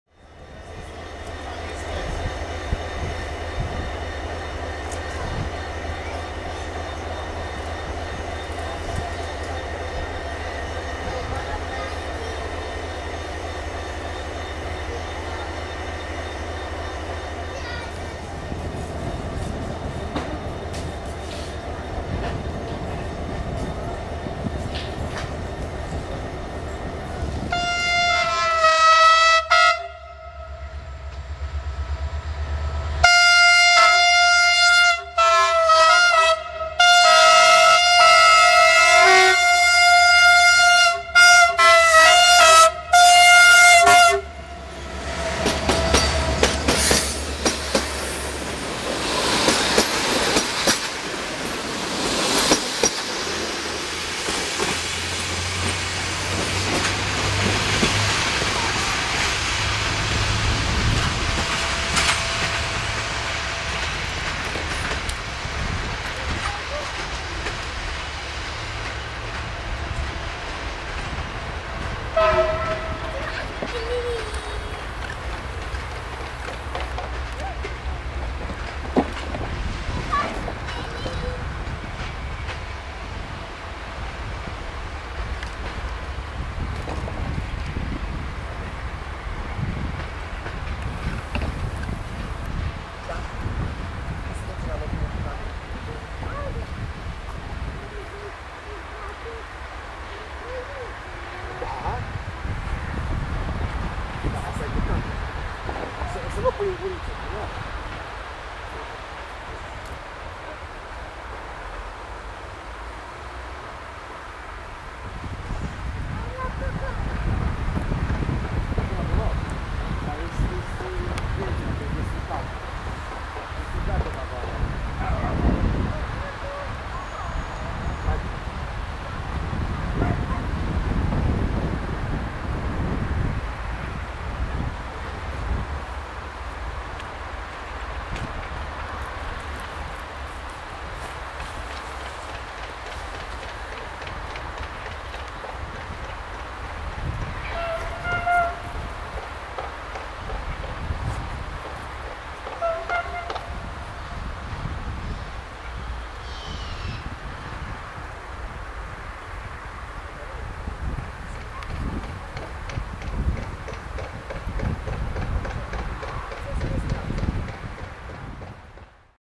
Jimmy EXPRESS cu Mecanic de Treaba in Oradea (06 09 2012)
Jimmy EXPRESS cu Mecnaic de Treaba in Oradea (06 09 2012)
Jimmy EXPRESS with Nice Runner in Oradea (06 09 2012)

1:Jimmy EXPRESS - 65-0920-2 - Asteapta Semnalul de plecare
2:R3118 - Oradea - Arad - Liber catre Oradea Vest
3:Video Ending